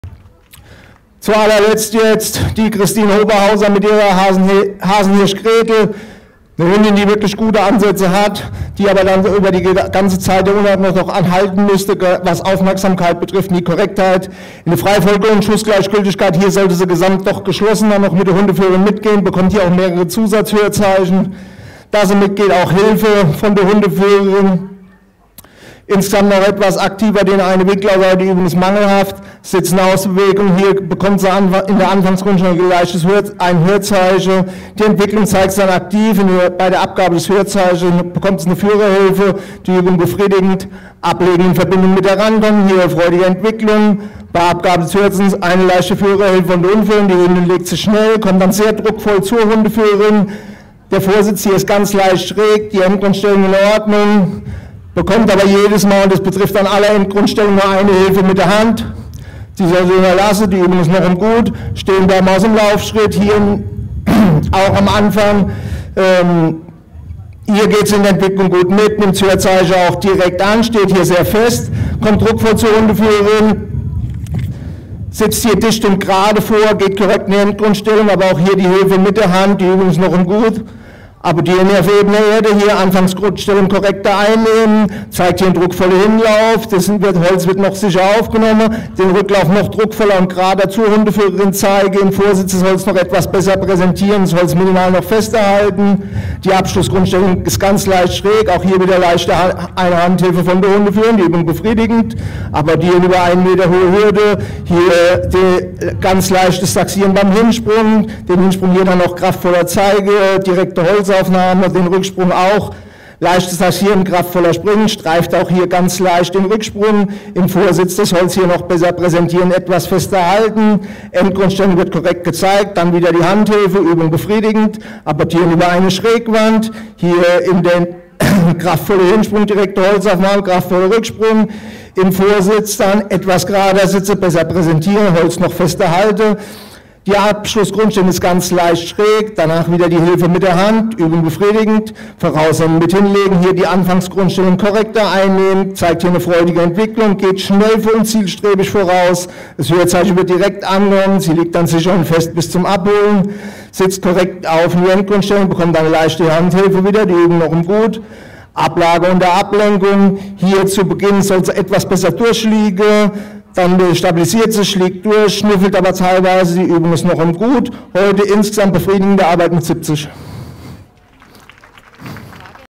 Hasenhirsch Gretel (5 Jahre) VDH DM IGP3 Abteilung B Unterordnung Richterbesprechung Marcus Kühn
Bouvier des Ardennes Hasenhirsch Gretel (5 Jahre) VDH DM IGP3 Abteilung B Unterordnung Richterbesprechung Marcus Kühn